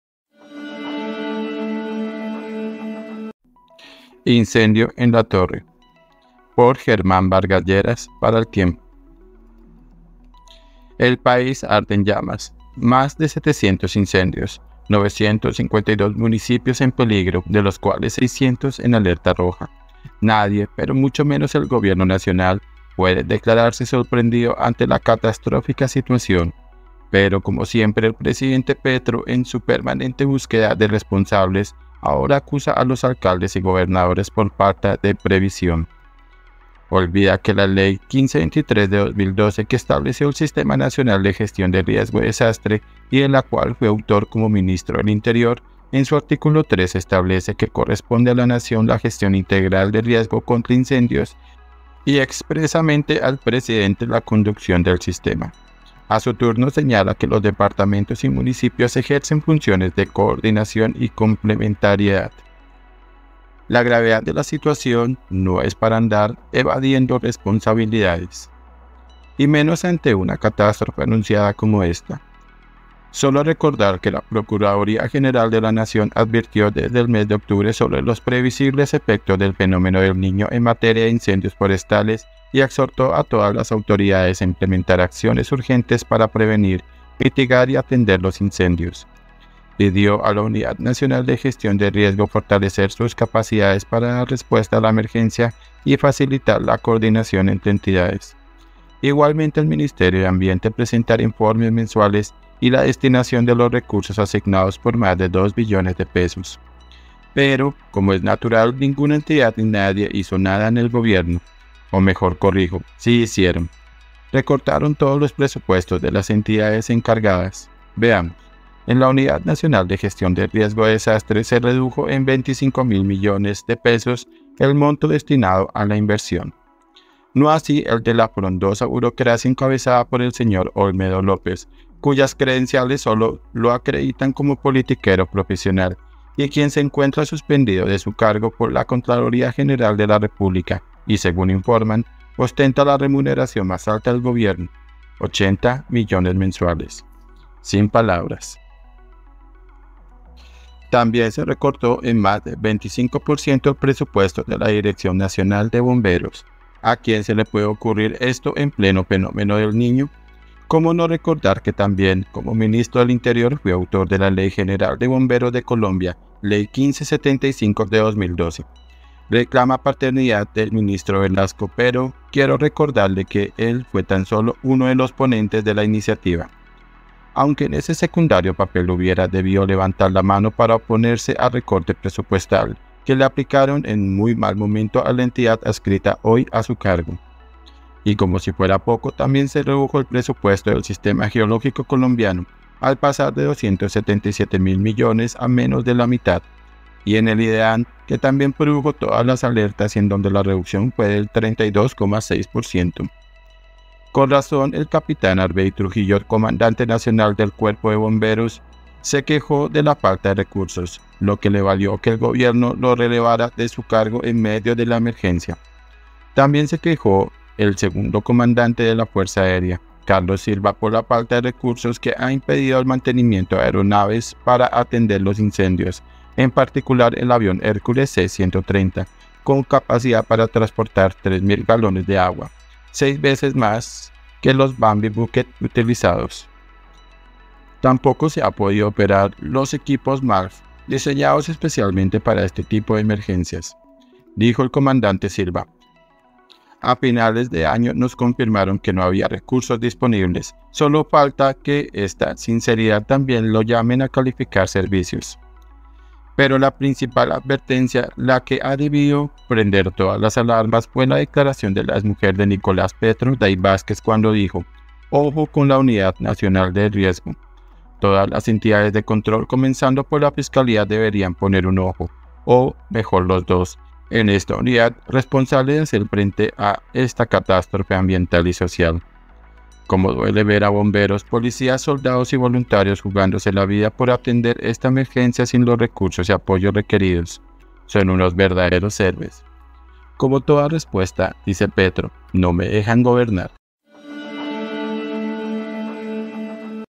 INCENDIO EN LA TORRE 🔴 Columna Germán Vargas Lleras
El país arde en llamas. Más de 700 incendios. 952 municipios en peligro, de los cuales 600 en alerta roja. Nadie, pero mucho menos el Gobierno Nacional, puede declararse sorprendido ante la catastrófica situación. Pero, como siempre, el presidente Petro en su permanente búsqueda de responsables ahora acusa a los alcaldes y gobernadores por falta de previsión.